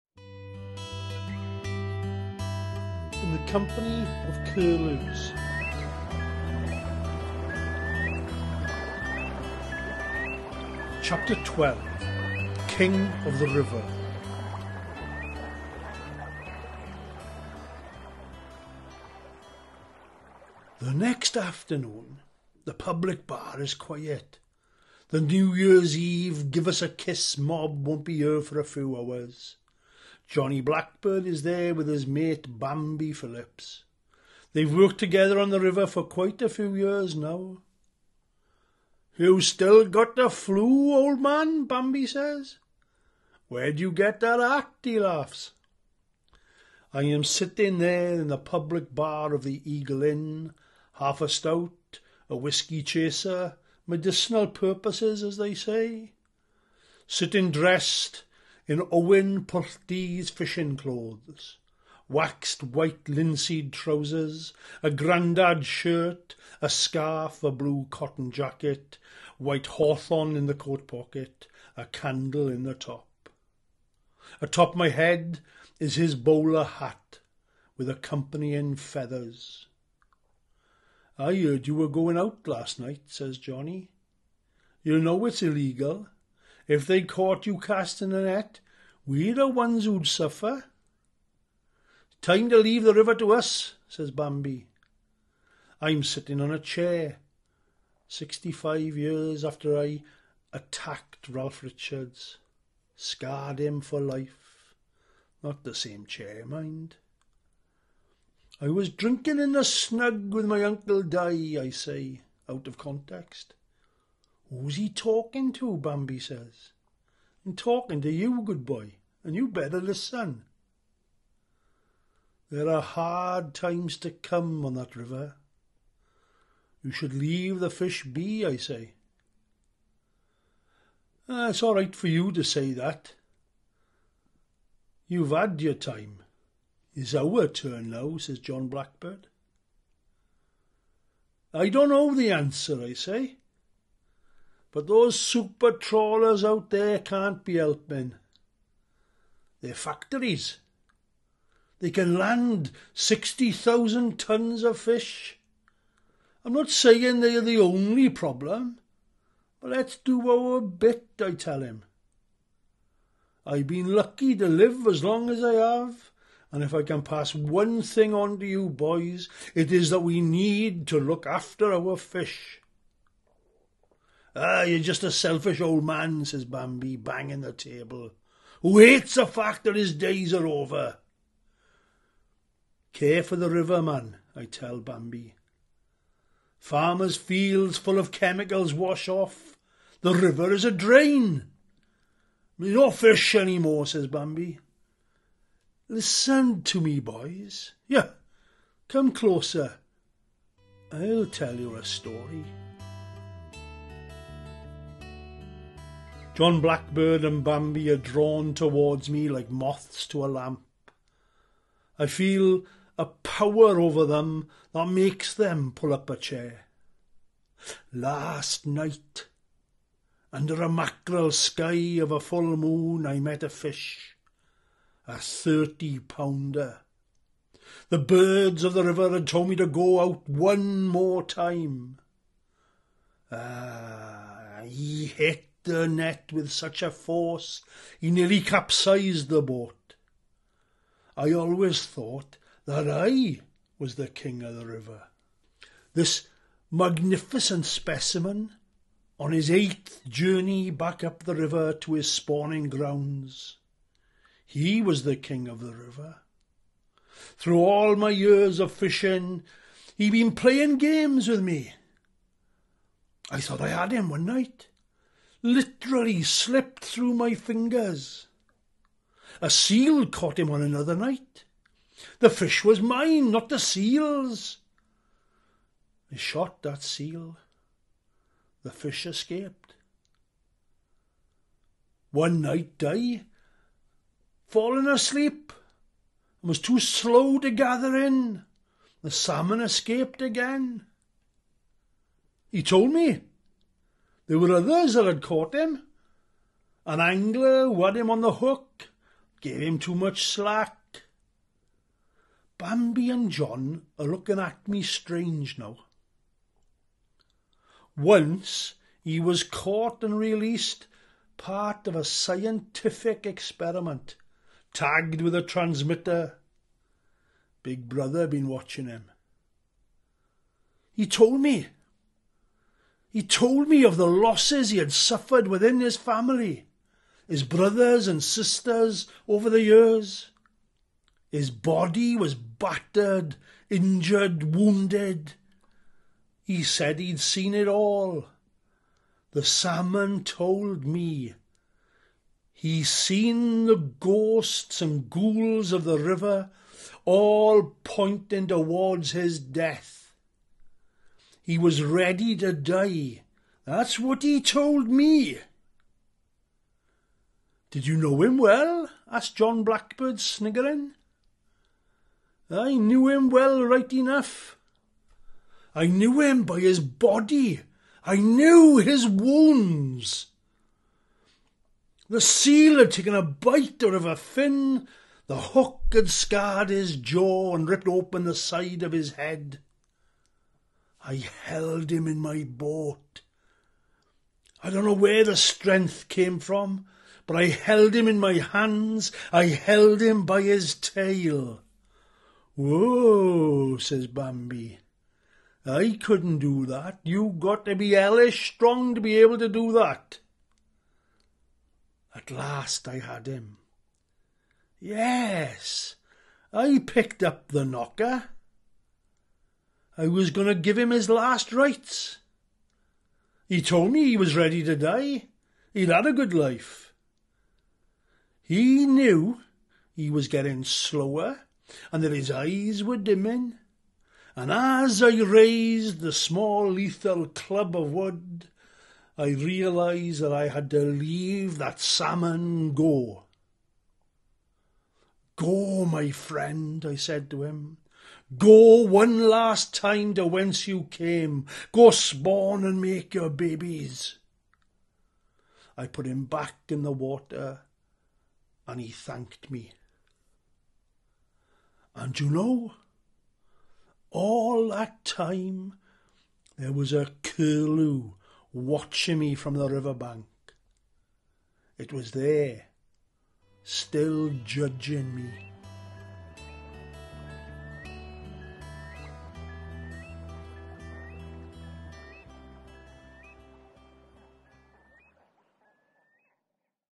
12 King of the River
Sin and redemption arc through the life story of a coracle man. During Jac’s young teenage life he makes the error of not speaking up for his sibling. He feels responsible for the tragic accident leading to his younger brother’s death. The guilt stays with him throughout his life as he makes mistake time and time again in his relationships with close family and friends.

The story incorporates the myths, tradition and stories of the river. One of the myths is the role of the Curlew. The bird in the coracle world is seen as a bird of wisdom and the judge of the river. It decides whether the fisherman is worthy. If found guilty for bad behaviour the curlew would banish the coracle man from the river never to catch a fish again.

When Jac finally satisfies his fishing appetite and catches the big fish he has sought all his life, he realises that it’s not his heart’s desire and returns it to the river to spawn again. His treatment of the fish is his atonement. He sees his brother again and although Jac is close to death himself he is at rest in his mind as he has resolved his issues of guilt.

The story ends with Jac showing an understanding of nature. He is then visited by a vision of his younger brother and other close family. They show forgiveness which allows Jac to go to his own death in peace.